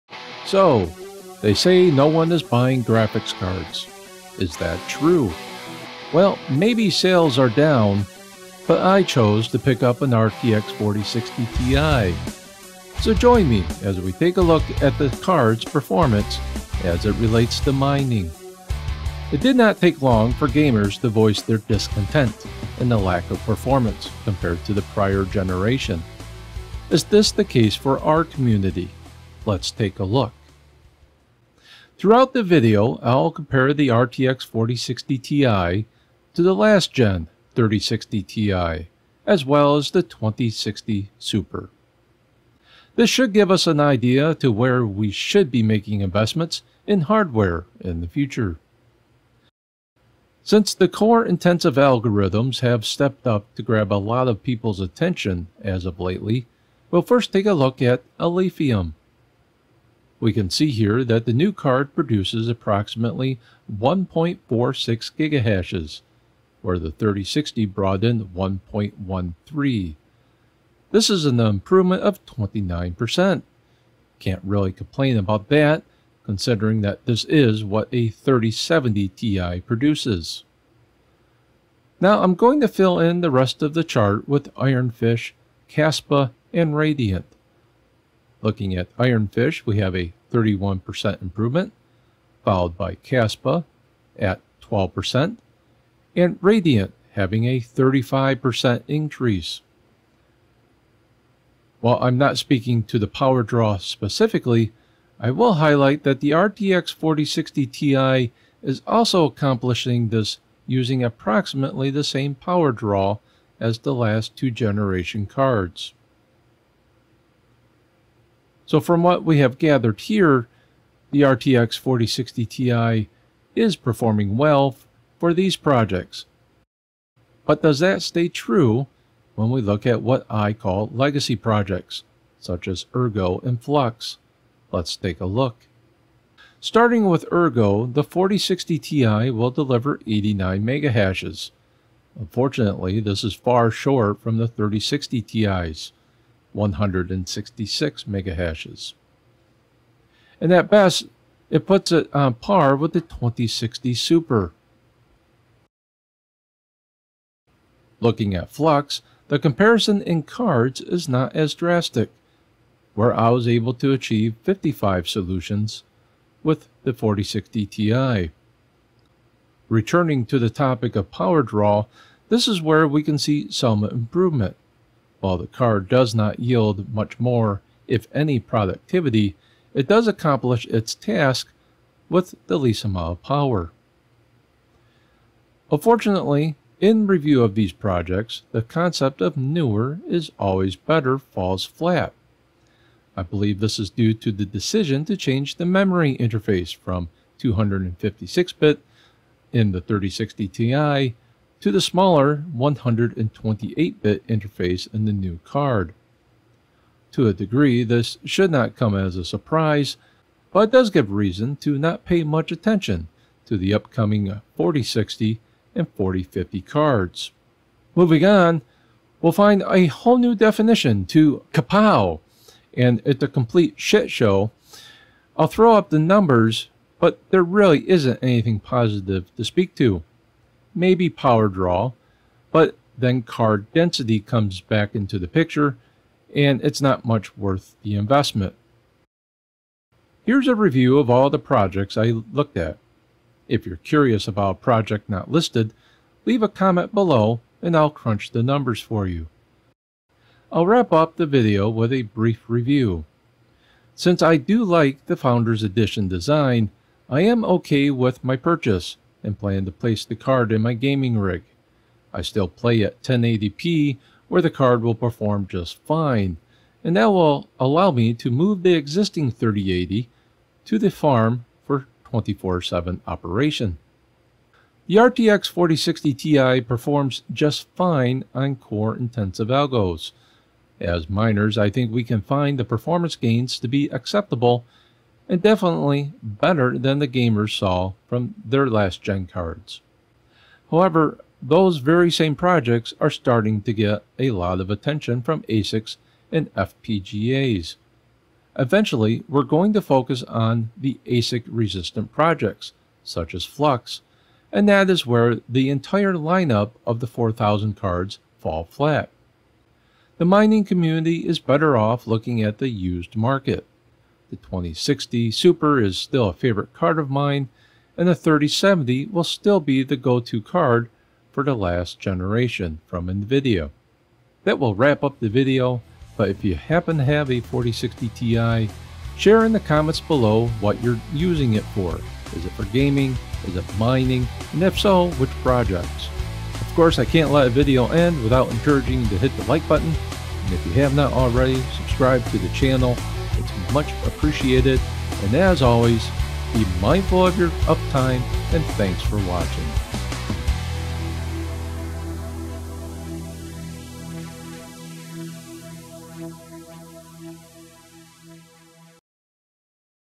A Miner's Preview Of The RTX 4060 Ti
Gamers are not happy, but what about miners?  Let's take a brief look at the performance of the RTX 4060 Ti when mining Alephium, Flux, Raven Coin and much more.